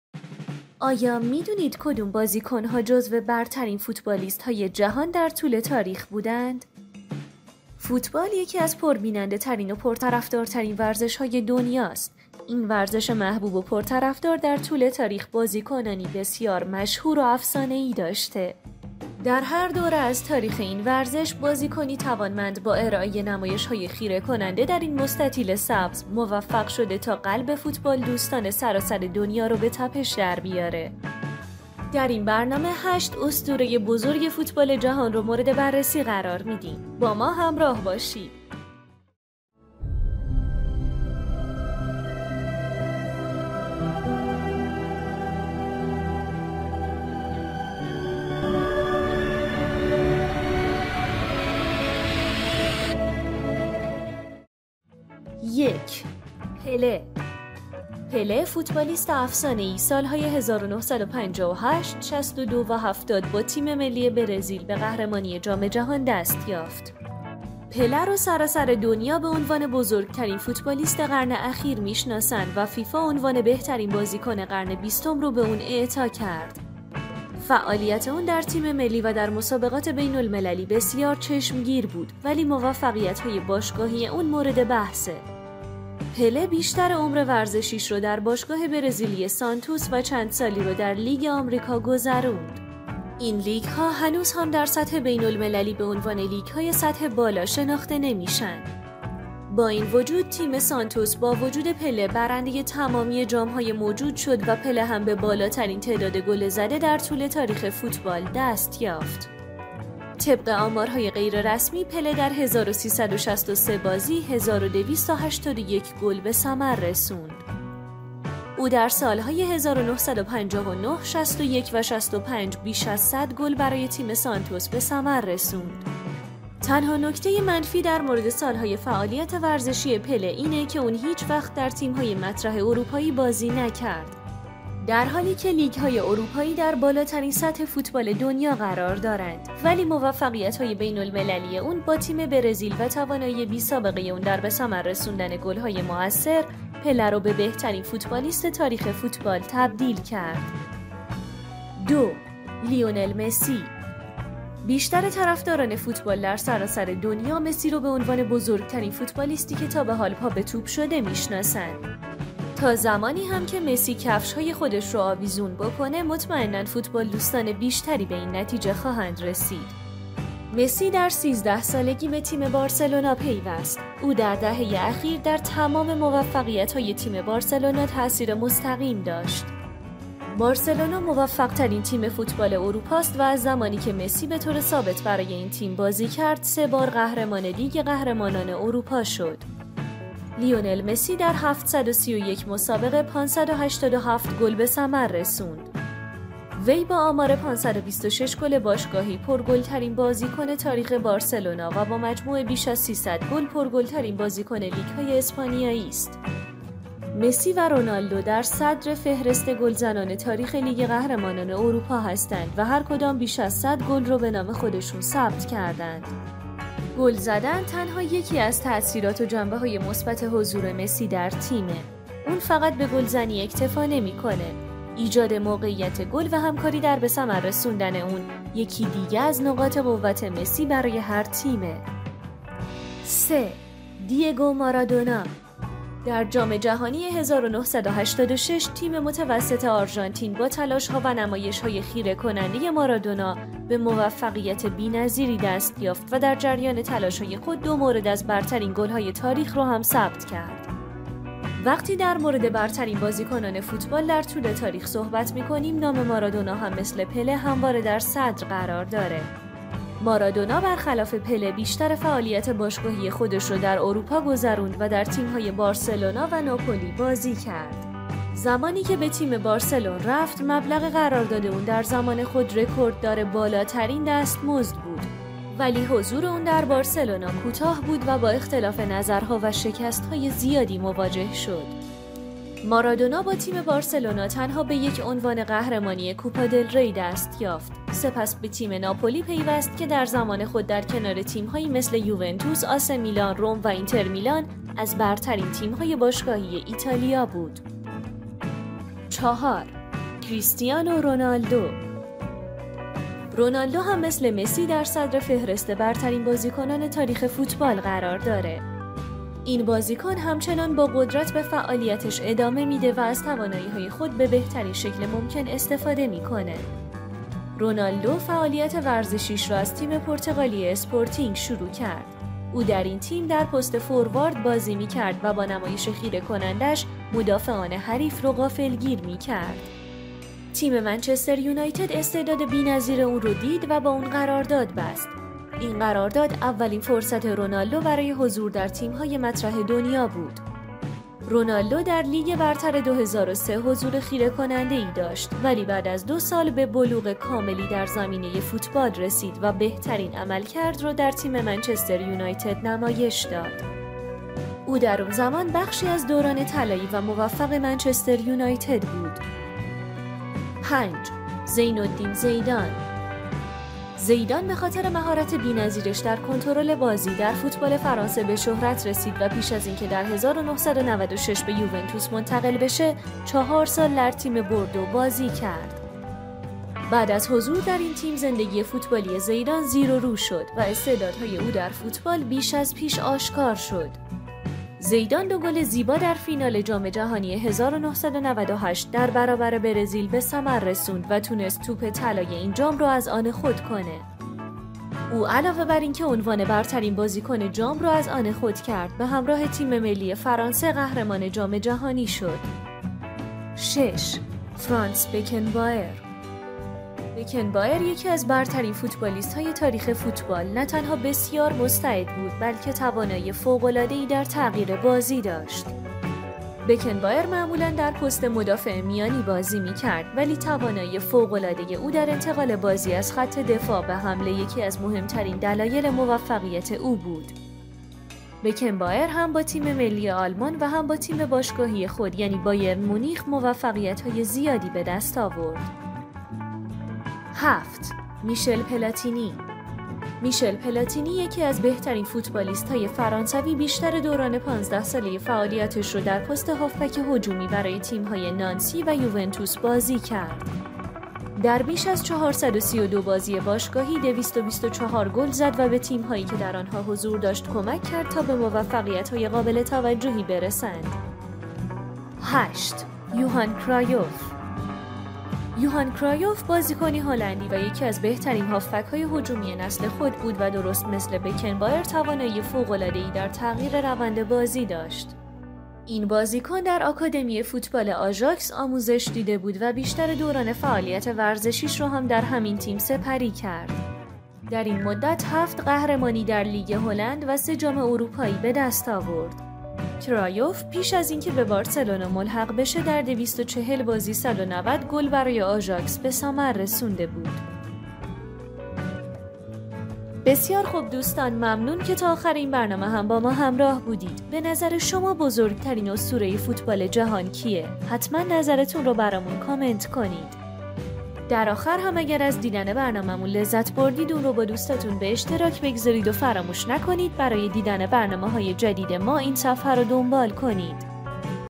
برترین فوتبالیست های جهان در طول تاریخ چه کسانی بودند ؟!؟
آیا می دانید چه افرادی در طول تاریخ بهترین های فوتبال بودند؟
بیشتر طرفداران فوتبال در سراسر جهان ، مسی را به‌عنوان بزرگ‌ ترین فوتبالیستی که تا به‌ حال پا‌به‌ توب شده است ، می‌شناسند و تا زمانی که مسی‌ کفش‌‌های‌ خود را آویز کند ، مطمئنا فوتبال‌ دوستان بیشتری به این نتیجه می‌رسند . لیونل مسی‌ در ۱۳ سالگی به گروه بارسلونا پیوست . او در دهه‌ی آخرین در تمام پیروزی های تیم بارسلونا ، تاثیر مستقیم داشته شده است . بارسلونا موفق‌ ترین باند فوتبال اروپا است و از زمانی که مسی به‌ طور مستقر برای این تیم بازی می کرد ، سه مرتبه قهرمان لیگ قهرمانان اروپا شد .لیونل مسی در ۷۳۱ آزمون ، ۵۸۷ گل به فایده رسانده خواهد بود . وی با آمار ۵۲۶ گل باشگاهی ، پر گل‌ ترین بازیکن تاریخ بارسلونا و با تمام بیش از ۳۰۰ گل ، پرگل‌ترین بازیکن لیگ‌های اسپانیایی است .
مسی و رونالدو در راس گروه گلزنان تاریخ لیگ قهرمانان اروپا قرار دارند و هر کدام بیش از ۱۰۰ گل را به نام خویش ثبت کرده‌ اند .

برترین های فوتبال
0:56 پله
2:32 لیونل مسی
3:54 مارادونا
5:00 رونالدو
6:01 زیدان
فرانس بکن بائر 6:44
یوهان کرایف 7:43

لطفاً برای حمایت از ما این صفحه را دنبال کنید و با لایک ها و نظرات مثبتتون این صفحه را همراهی کنید